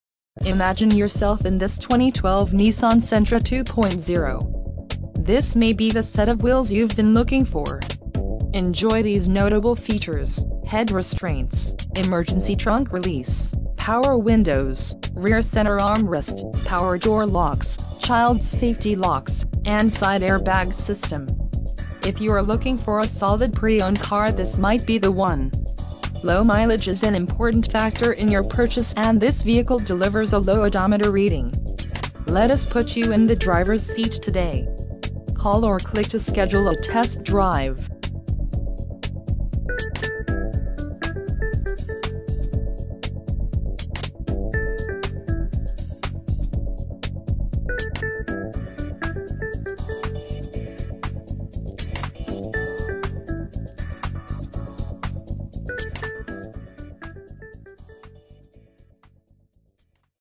2012 Nissan Sentra - Ivory Chevrolet - Union City, GA 30291
Imagine yourself in this 2012 Nissan Sentra 2.0. This may be the set of wheels you've been looking for. Enjoy these notable features: Head Restraints, Emergency Trunk Release, Power Windows, Rear Center Armrest, Power Door Locks, Child Safety Locks, and Side Air Bag System. If you are looking for a solid pre-owned car this might be the one. Low mileage is an important factor in your purchase and this vehicle delivers a low odometer reading. Let us put you in the drivers seat today.